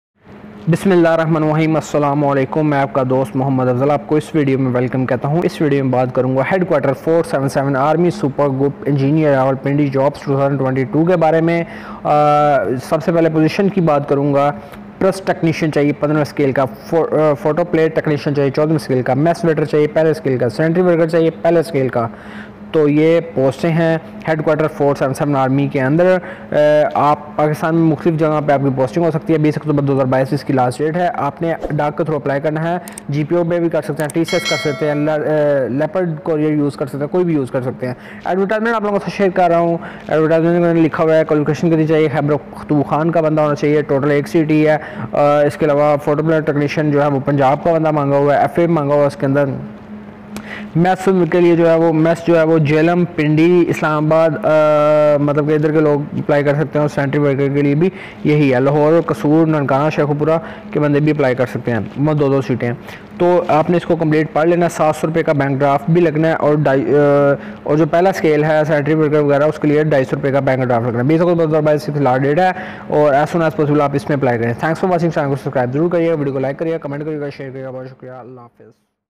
Headquarter 477 Army Survey Group Engineers Rawalpindi Jobs 2022 || Pak Online Jobs
Positions:
Press Technician (BPS-15)
Photoplate Technician (BPS-14)
Mess Waiter (BPS-01)
Sanitary Worker (BPS-01)

City / Location:
Khyber Pakhtunkhwa (KPK) , Punjab, Jhelum , Rawalpindi , Islamabad , Attock , Hazara , AJK , Lahore , Kasur , Nankana Sahib , Sheikhupura
Cleaner , Sweeper , Khakroob

Application Deadline:
20 Oct 2022